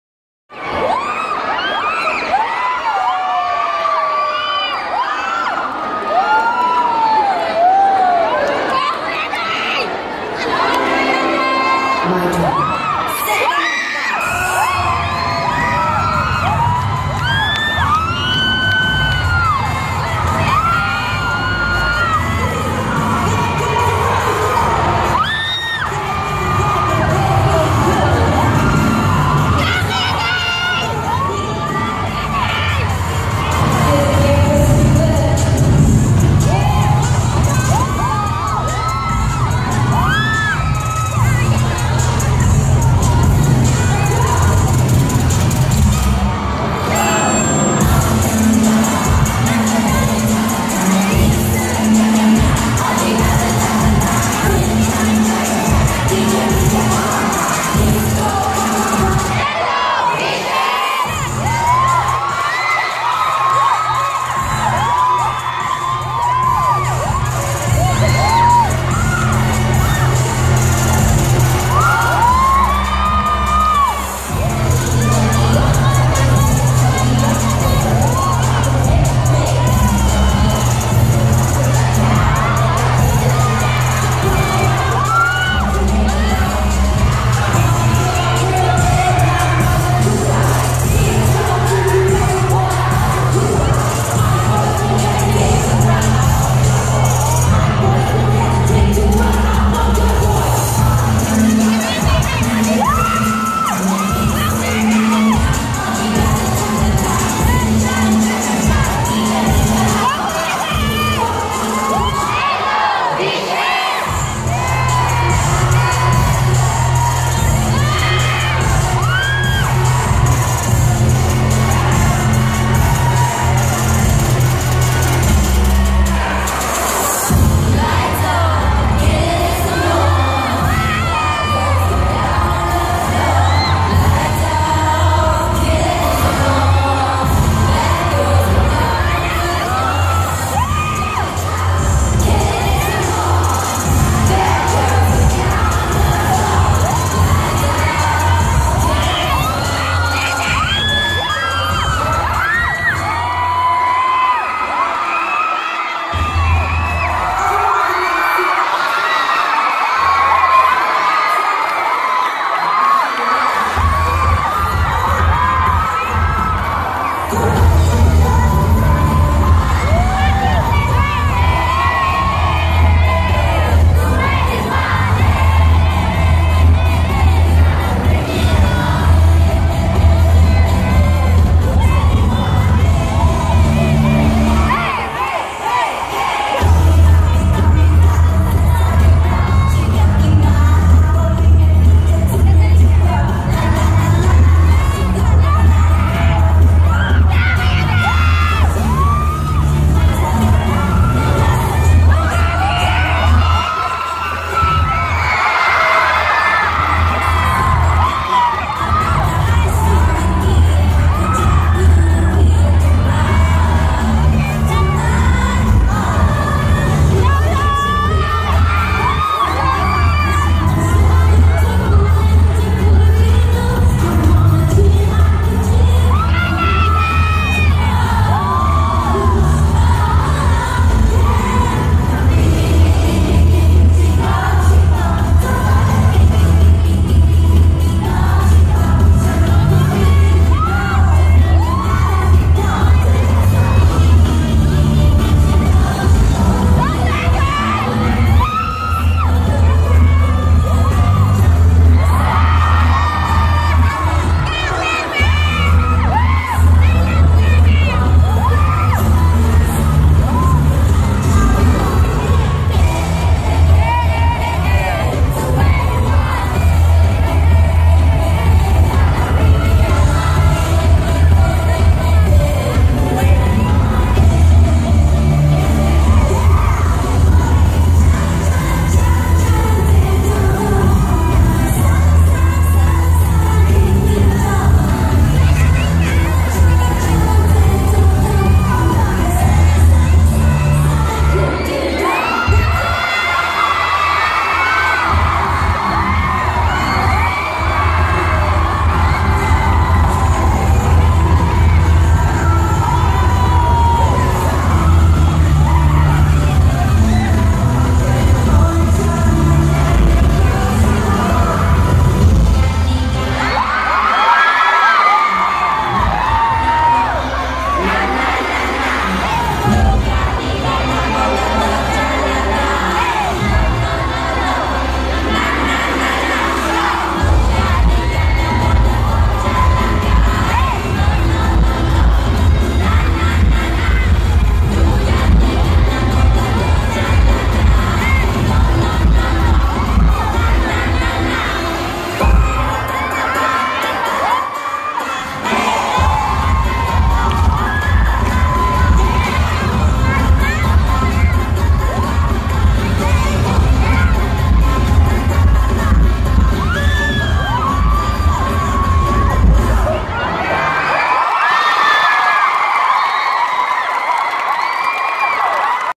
reQuest at HAPPY HALLYU DAY (Skydome)
reQuest Covering 2ne1 at HAPPY HALLYU DAY.. Sm north Skydome and one of Opening Act this coming December 2016 at Philippine Kpop Convention year 8.